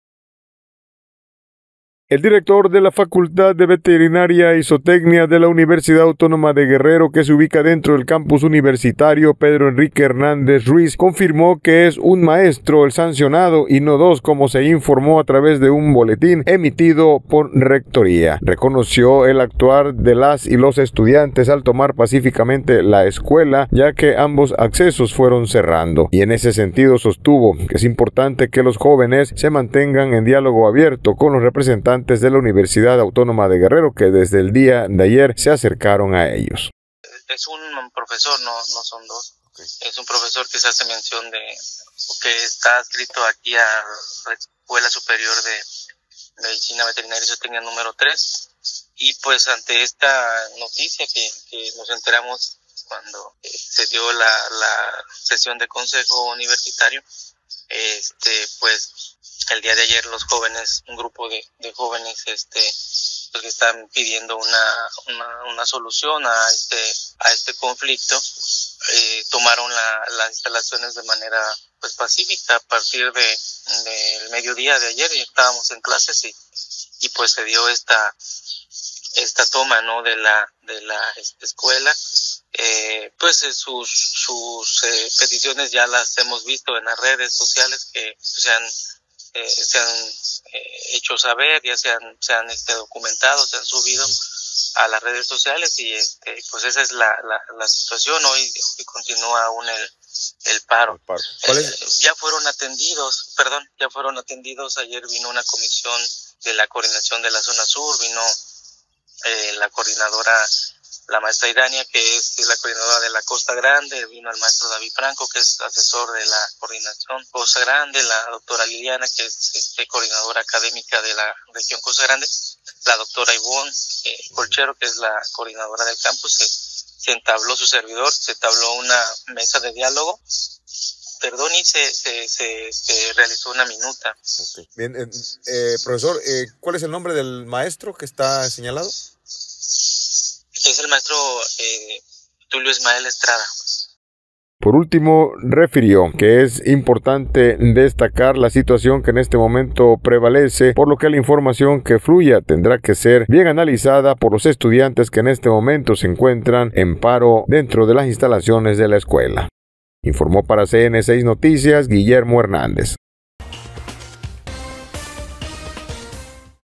Corrección: Un maestro sancionado, no dos, confirma director de la Facultad de Veterinaria
Pedro Enrique Hernández Ruiz director de la facultad de veterinaria  confirmo que es un maestro sancionado y no dos como se informo